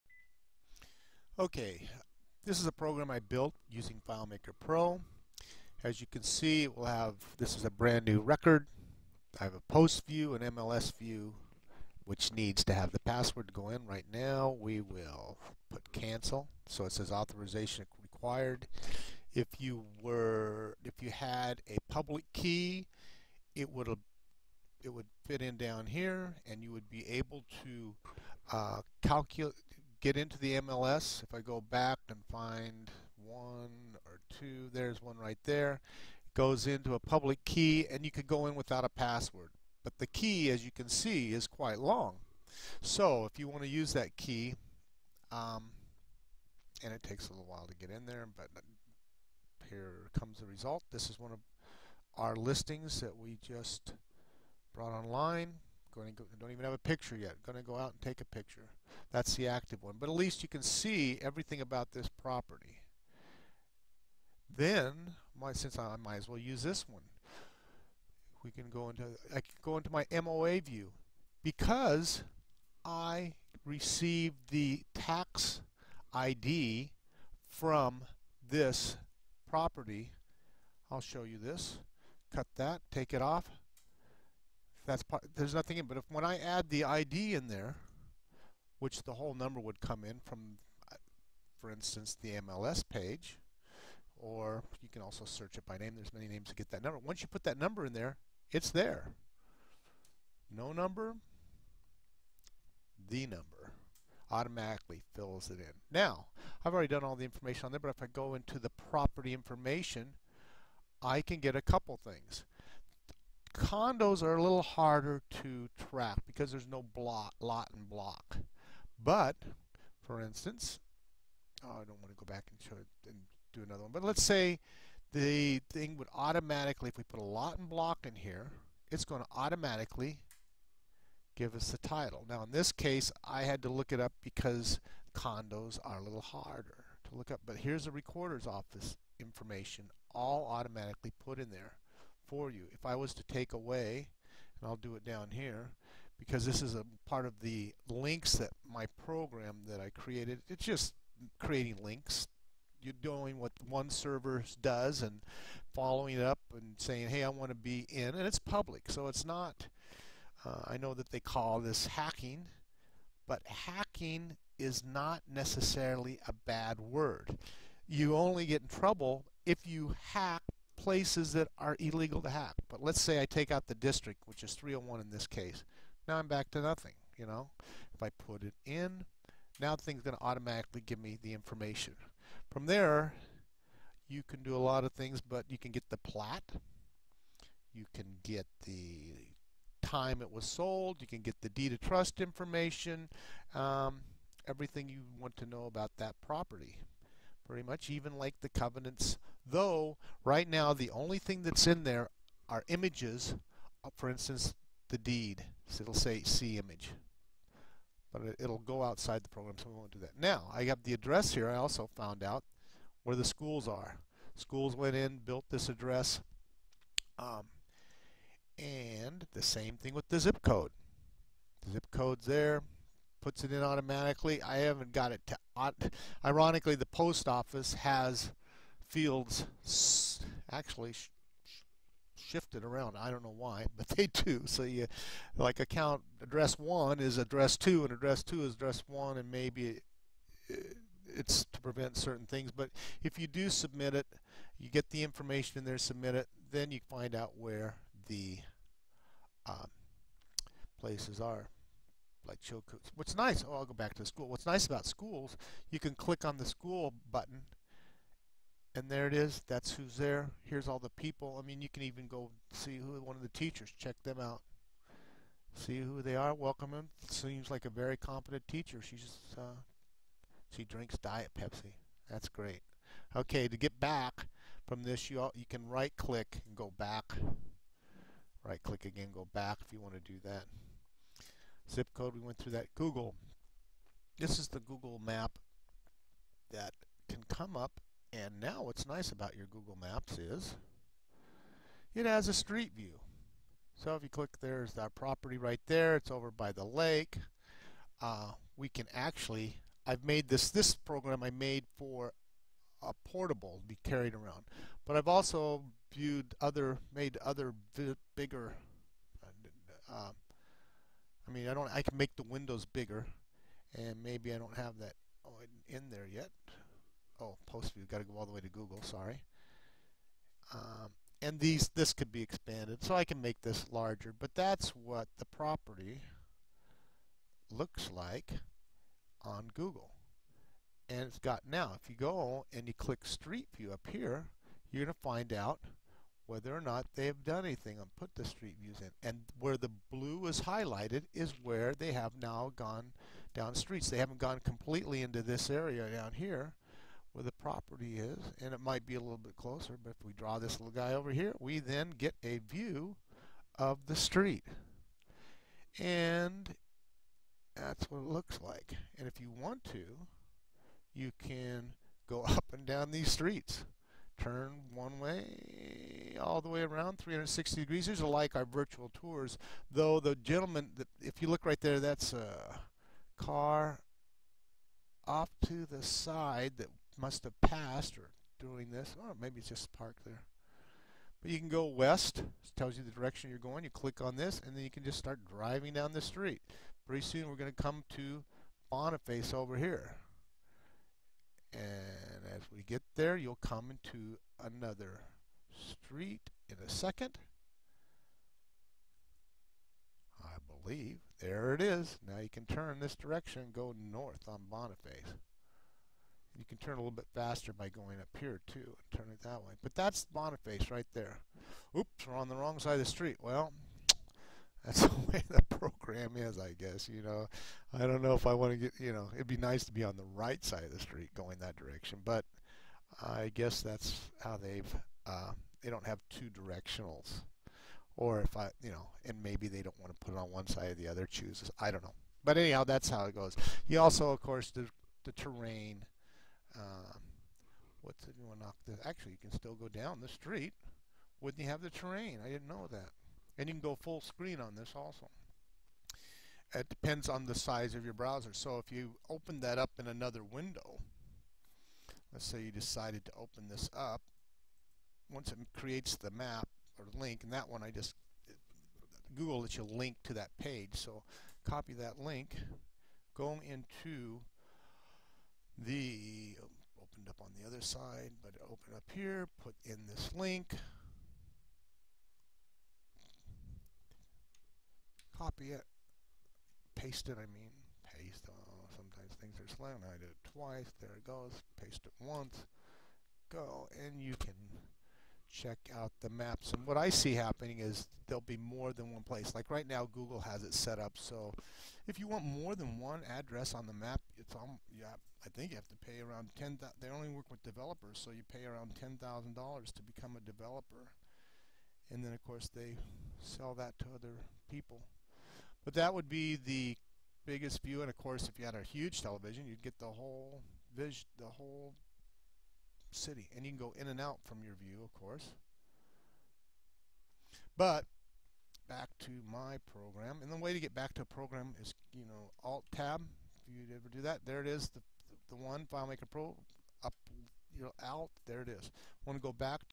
How I use Craig's List Part 2a Plus Foreclosures MLS etc.
Using Filemaker Pro I have created a program that builds on the link before it and relates all of it together so I never have to look up that information again. I also use the same concept to help my 3 YO granddaughter catagorize her information.